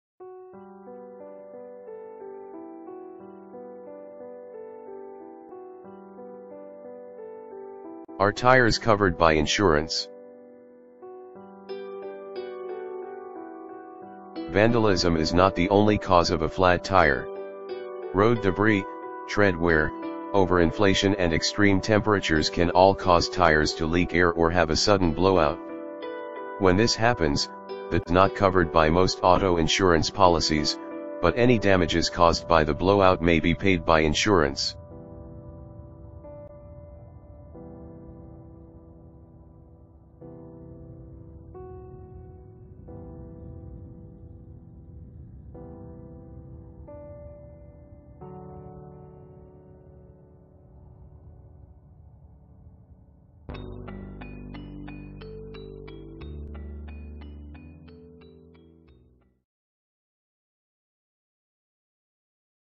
Are Tires Covered By Insurance
However, insurance companies will cover damage to your vehicle if a blowout or flat tire is the culprit. For example, if you tire blows out and causes damage to the body of your vehicle, you should be able to file a claim if you have comprehensive coverage.However, it does not cover wear and tear to a car or its tires due to bad road conditions. Collision coverage is generally sold with a deductible—the higher your deductible, the lower your premium. ... Potholes usually cause relatively minor damage to cars, mostly to the wheels and tires.If it's damage to the actual axle, body or other parts that aren't the wheels. ... In this case, some insurers might cover all damage, including tires and wheels, others might cover all damage except the tires and others still might pay for the cost of repairs minus the cost of existing wear and tear.Yes, according to the Insurance Information Institute, some damages caused by a pothole are covered if you have collision coverage. Collision coverage is an optional coverage of your auto policy. ... Your auto insurance policy also covers you if you hit another car or a pedestrian as a result of the pothole.